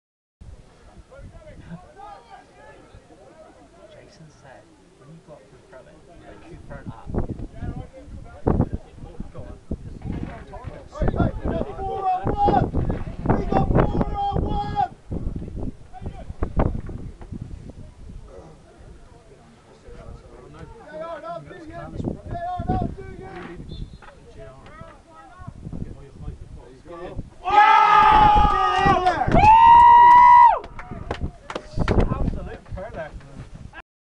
The Bond Timber CCFA Junior Cup Semi-Final,St Stephen 5 - 2 Penryn 3rds
Courtney Rowe makes it 4-1 with this free-kick from 20 yards.